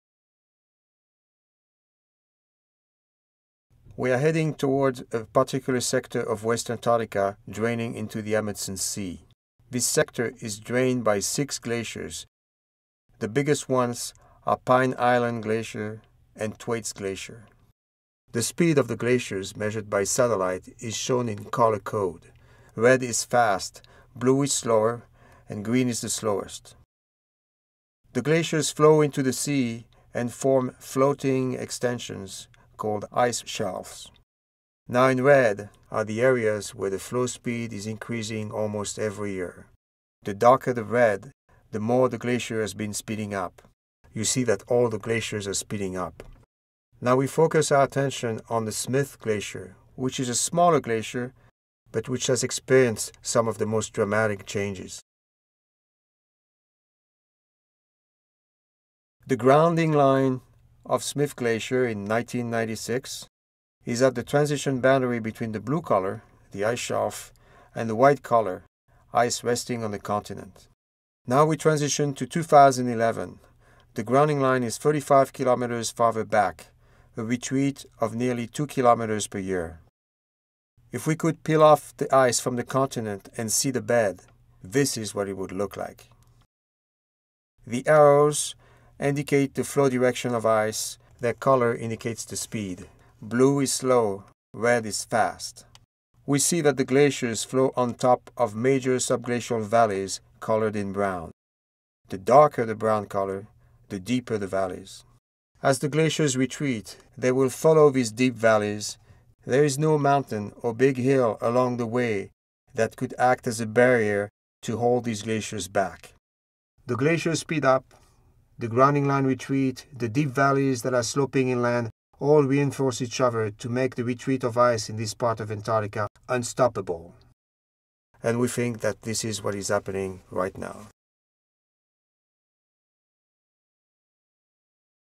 Runaway Glaciers in West Antarctica
Glaciologist Eric Rignot of NASA's Jet Propulsion Laboratory and the University of California, Irvine, narrates this animation depicting the processes leading to the decline of six rapidly melting glaciers in West Antarctica.  A new study by Rignot and others finds the rapidly melting section of the West Antarctic Ice Sheet appears to be in an irreversible state of decline, with nothing to stop the glaciers in this area from melting into the sea.